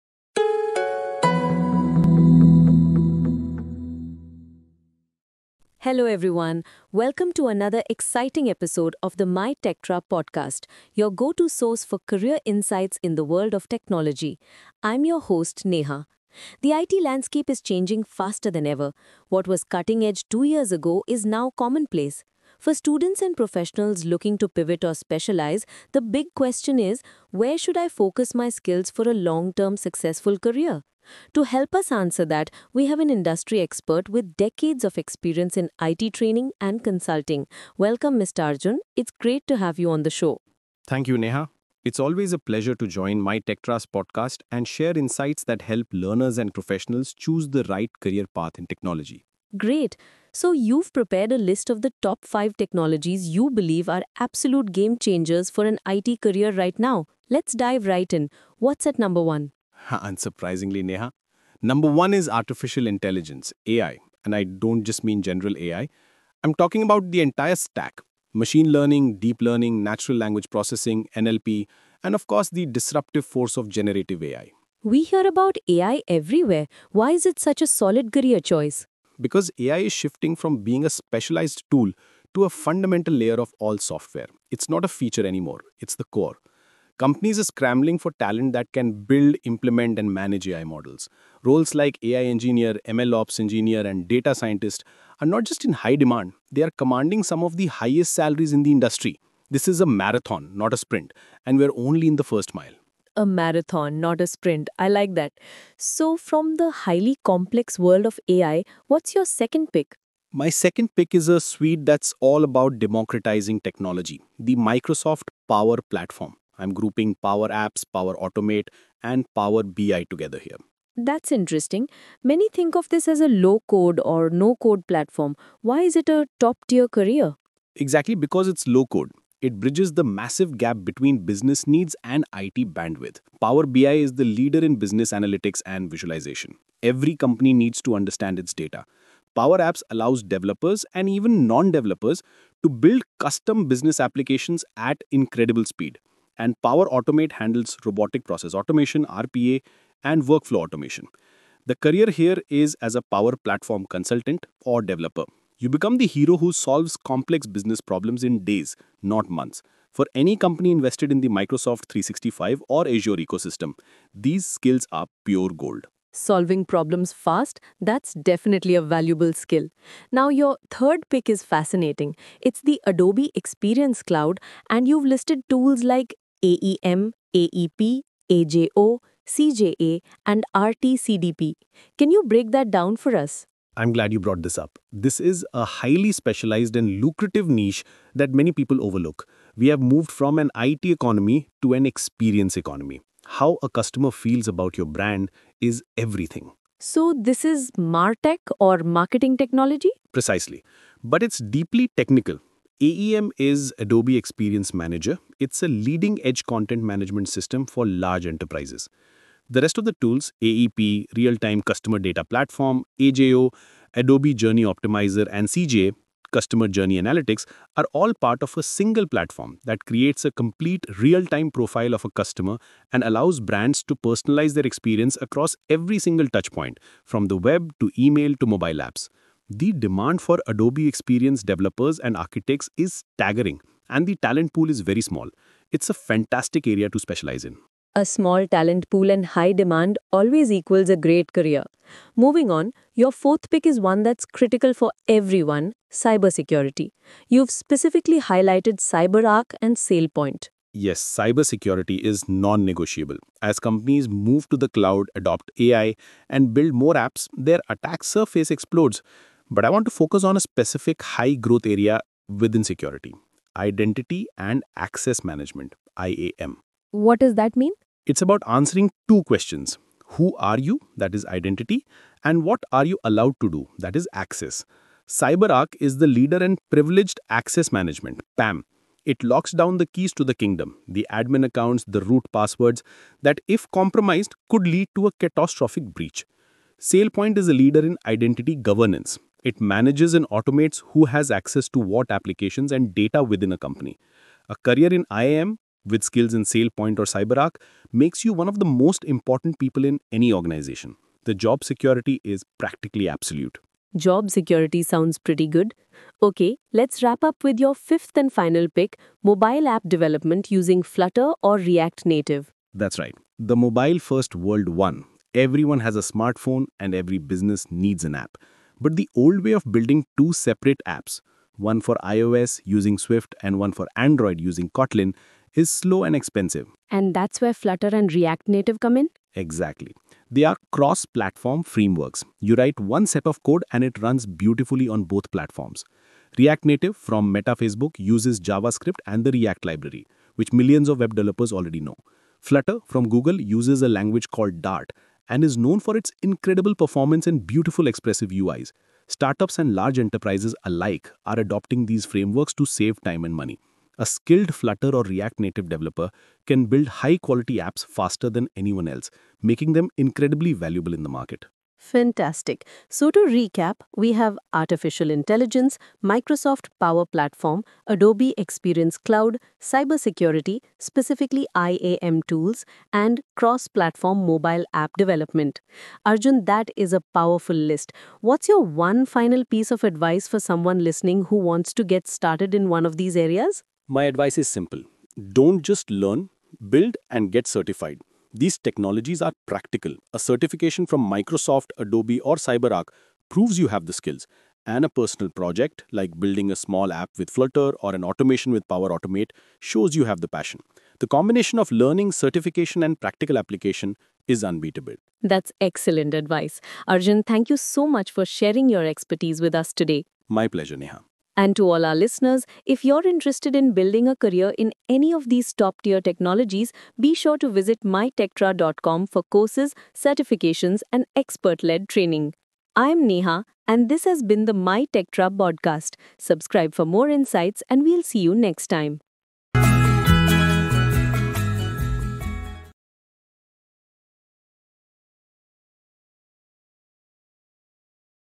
Top 5 Technologies to Choose for an IT Career | myTectra Podcast
In this episode of the myTectra TechTalk Podcast, we dive deep into the Top 5 Trending Technologies that are transforming the IT landscape in 2025 — and how YOU can upskill to stay ahead in your career! 🚀

Whether you’re a student, IT professional, or career switcher, this episode gives you expert insights into where the tech industry is heading — and which training programs can help you future-proof your skills.

🔹 #1. Artificial Intelligence (AI)
Learn why AI remains the backbone of digital transformation — from Machine Learning and NLP to Generative AI and Automation.

🔹 #2. Microsoft Power Platform (Power Apps, Power Automate, Power BI)
Empower your career with low-code development, process automation, and business intelligence using the Power Platform ecosystem.

🔹 #3. Adobe Experience Cloud Tools (AEM, AEP, AJO, CJA, RT-CDP)
Master customer experience management and digital marketing automation with Adobe’s suite of enterprise tools.

🔹 #4. Cybersecurity Tools (CyberArk, SailPoint)
Explore the most in-demand cybersecurity domains like Identity & Access Management, Threat Detection, and Data Protection.

🔹 #5. Mobile App Development (Flutter, React Native)
Build cross-platform mobile apps efficiently and become a full-stack mobile developer with the latest frameworks.